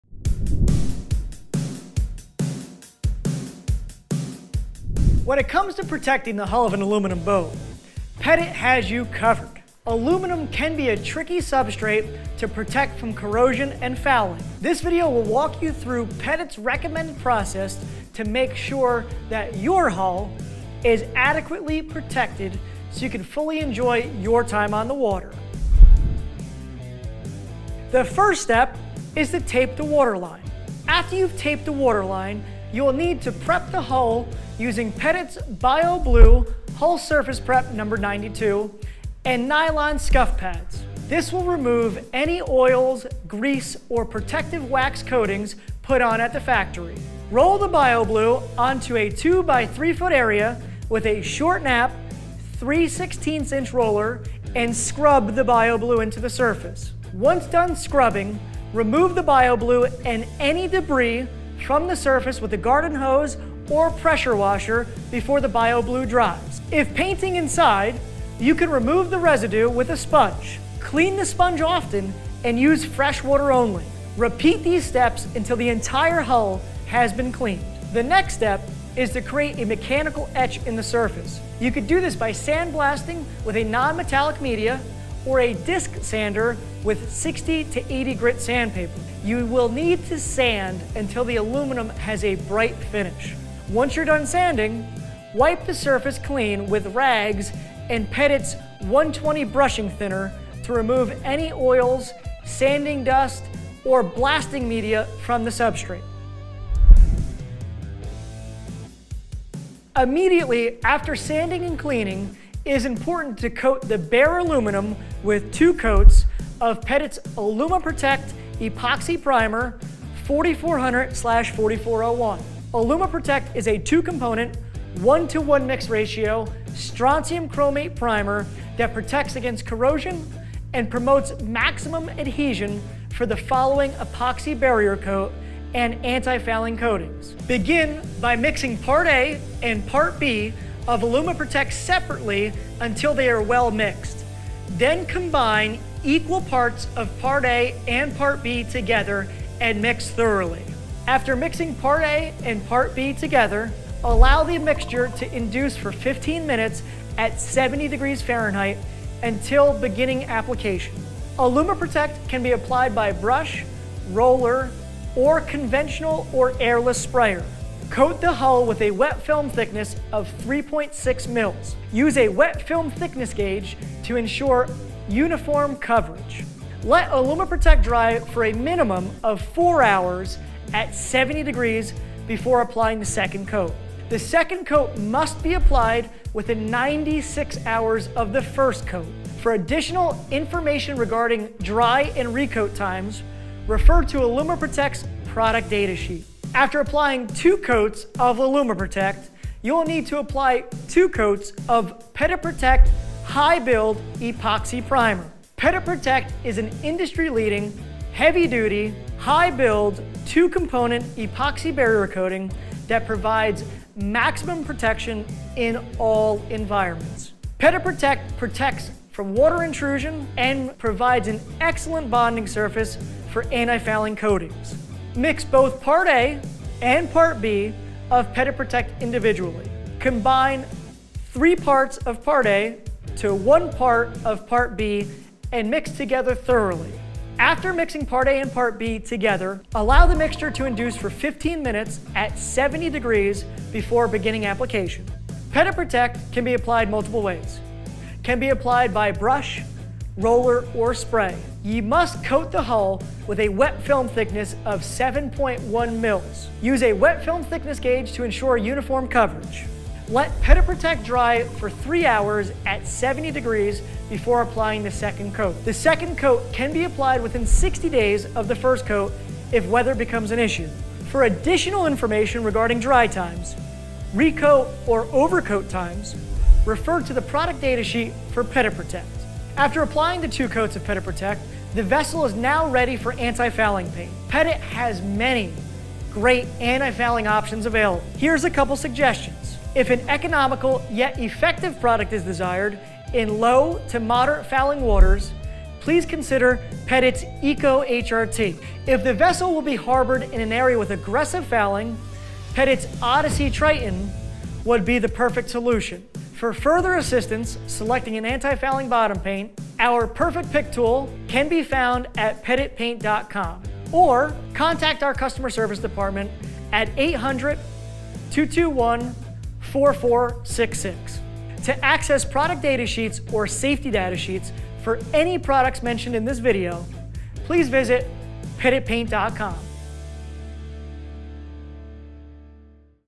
Pettit Paint - How To Antifouling Bottom Paint an Aluminum Boat
This video provides step by step instructions on how to apply Pettit products to the bottom of an aluminum hull such as a pontoon boat to prevent corrosion and fouling in fresh or saltwater.